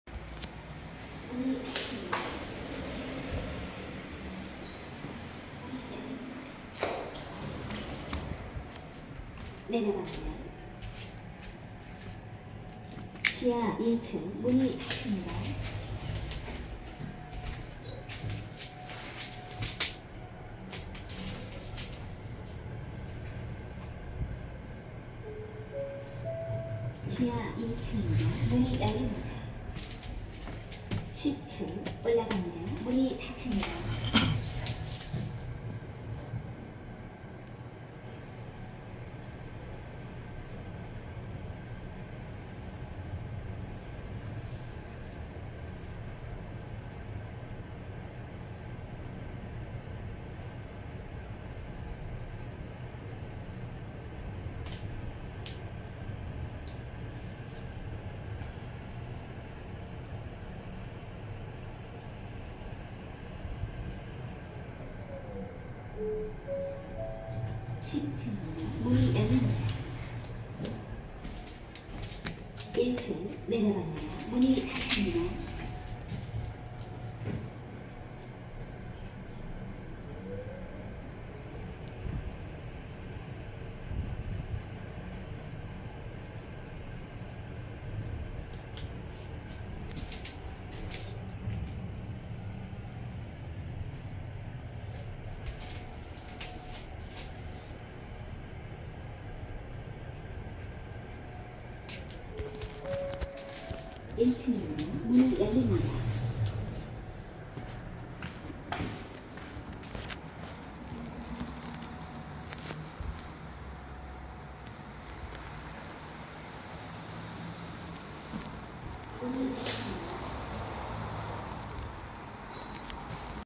충청북도 청주시 흥덕구 오송읍 봉산리 테라스퀘어9 OTIS엘리베이터 탑사기
2024년 7월 21일 영상분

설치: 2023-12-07
모델: ACD4MRLGR(M)
종류: 소방구조/장애인용
속도: 105m/min